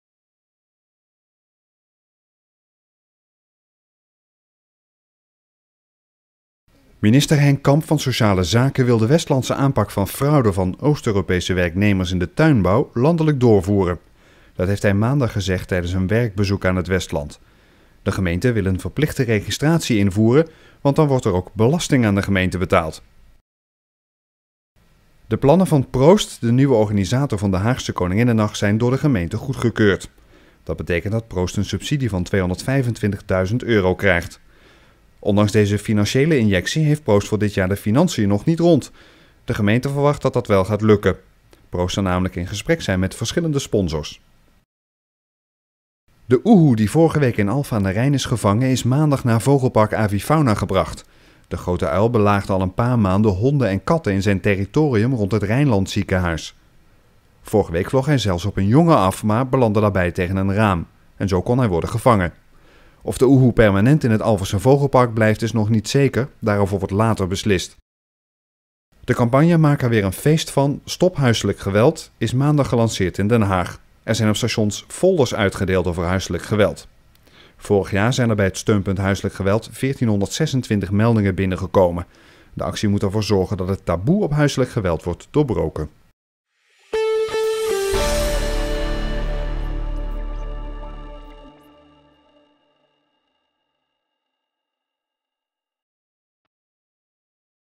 West in 90 seconden, maandag 14 februari - 17.00 uur
* Minister wil landelijk met Westlandse aanpak illegale werknemers
* Plannen voor nieuwe Haagse Koninginnenach goedgekeurd
* Avifauna is een oehoe rijker
* Actie tegen huiselijk geweld in Den Haag